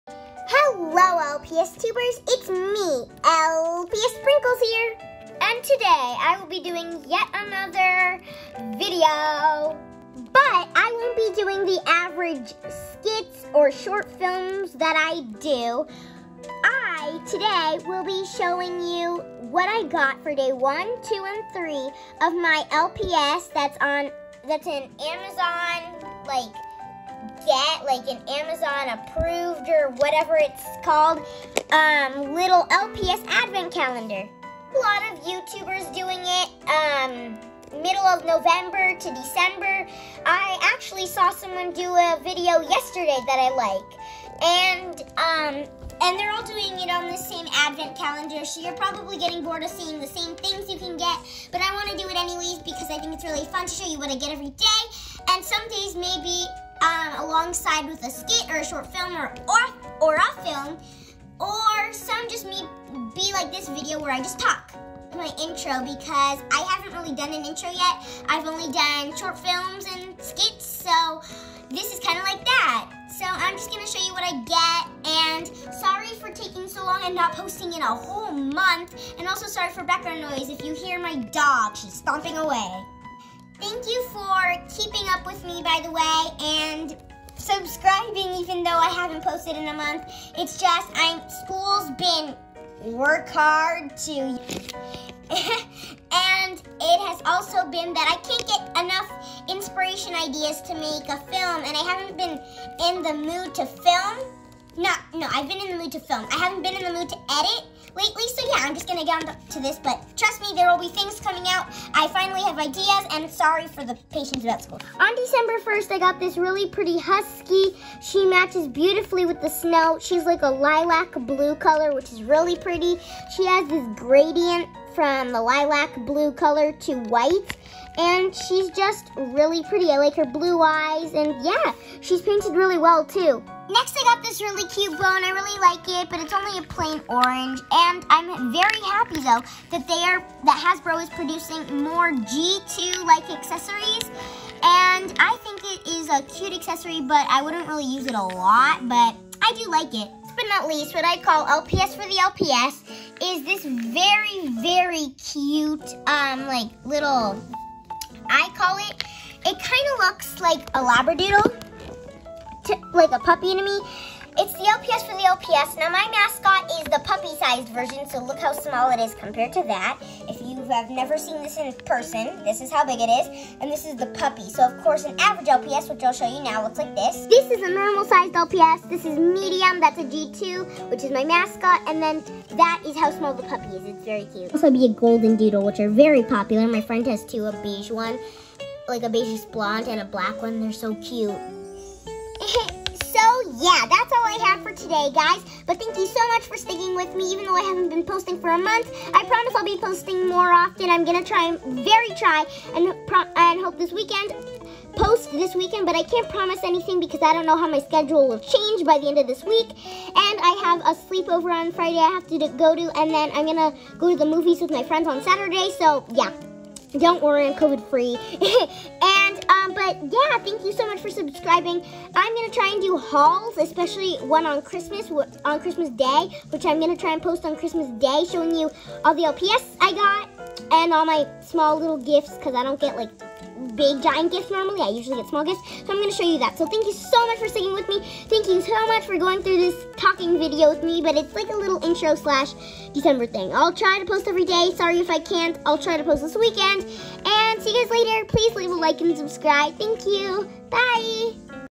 LPS: Advent Calendar day 1-3
Hi. I know I haven’t been posting but I’m gonna try to post more often! I haven’t been in the “inspiration” zone yet but I have great ideas now and I’m ready to use them! Expect more videos more often!! Sorry I speed talk. Thanks 💖Created by InShot